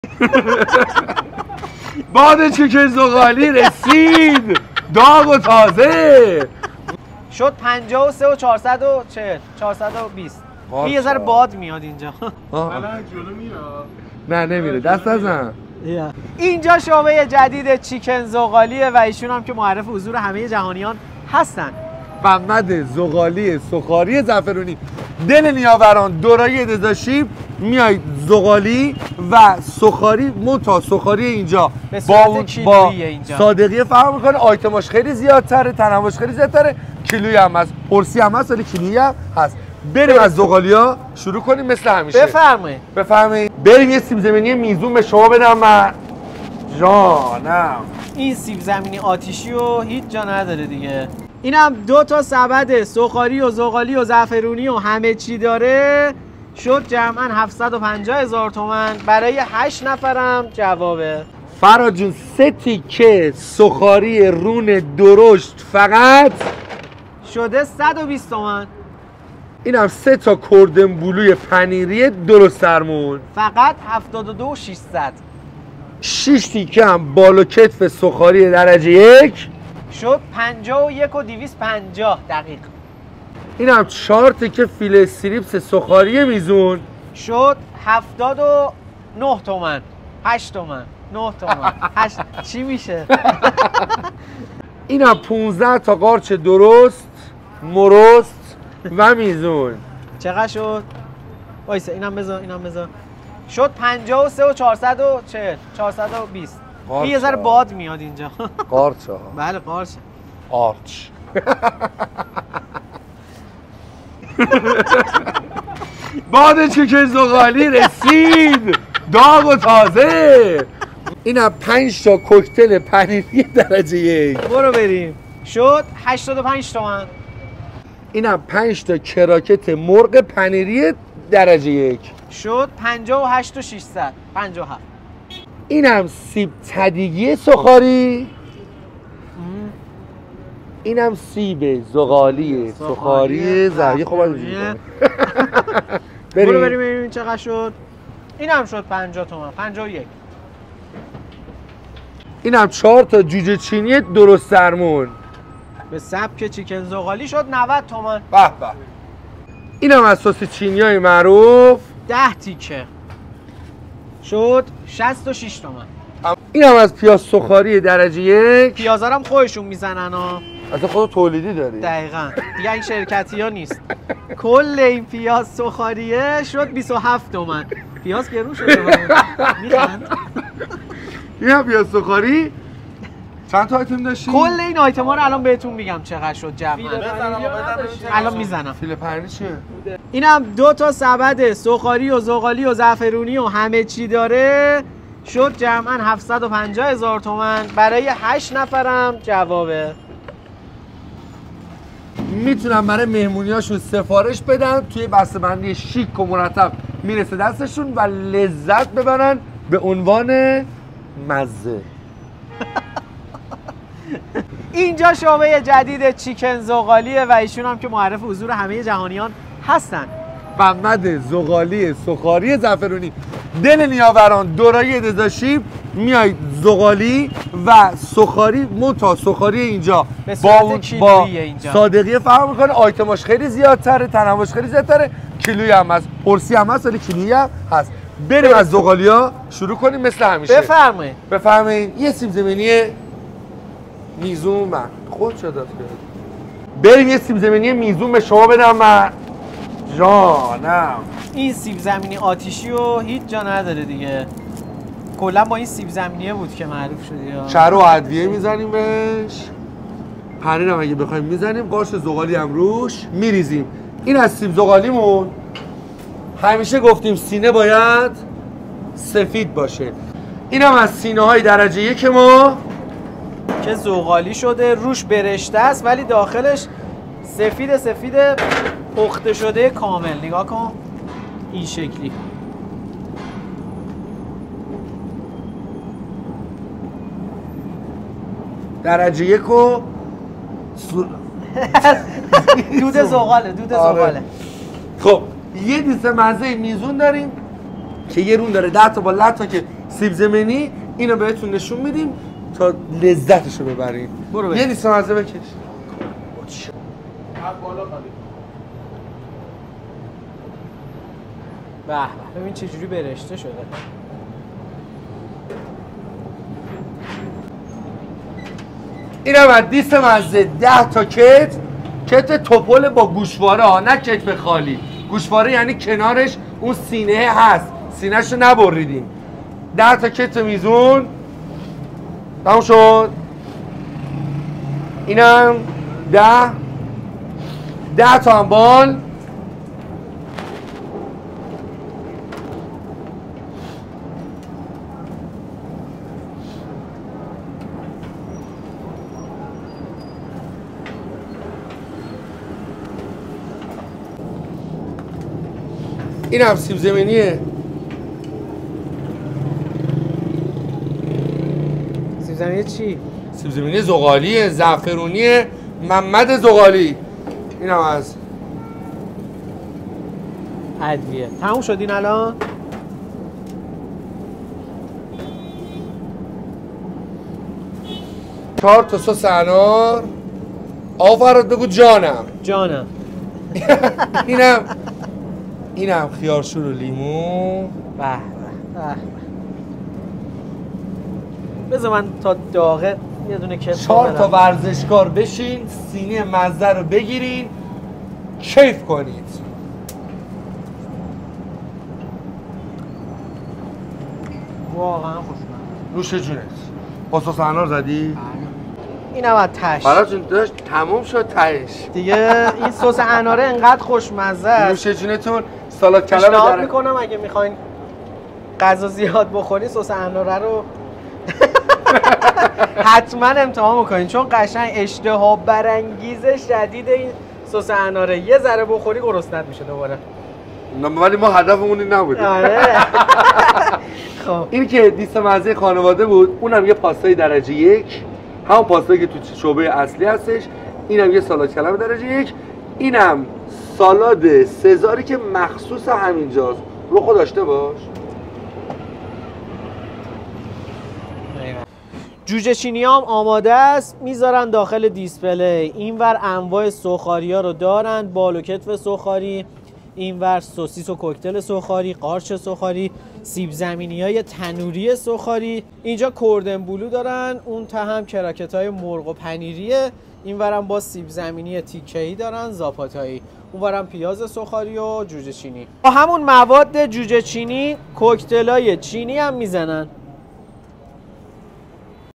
شعبه دوم چیکن ذغالی تو خیابان دزاشیب | Chicken Zoghali
اومدم شعبه دوم چیکن ذغالی تو خیابان دزاشیب

.چند وقته که افتتاح شده و یه کار جالبی که اینجا کردن اینه که انواع سوخاری رو به صورت کیلویی و حتی دونه ای میفروشن و شما میتونین از هر ایتم سوخاریشون هر چنتا میخواین انتخاب کنید
.کتف و بال ذغالیشون که بینظیره هیچ جا واقعا این طعم و خوشمزگیرو نداره

ماهی قزل آلا ذغالی و سیب زمینی زعفرونی ذغالیشون جزو بهتریناست من خودم همیشه مشتری ماهی هاشونم
.واسه مهمونیها و دورهمیاتون حتما بهشون زنگ بزنید کارتونو راه میندازن😉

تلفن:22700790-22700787
آدرس:نیاوران،خیابان باهنر،نبش سه راه نیاوران